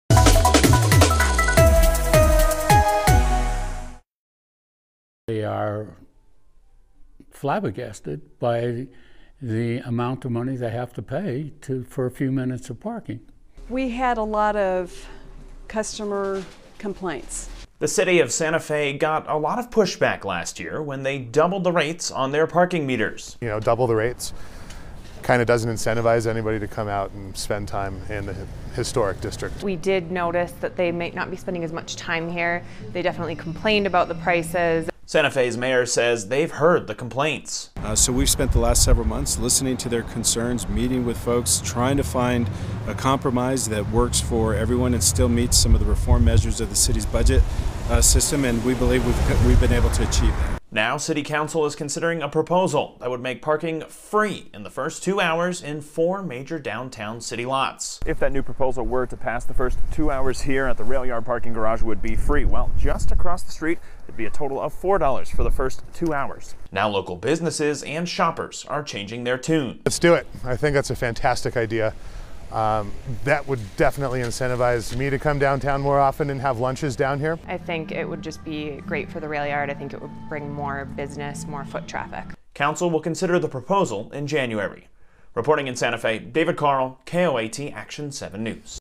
Santa Fe locals thrilled about free parking proposal
The new proposal would make first two hours of parking free in four lots.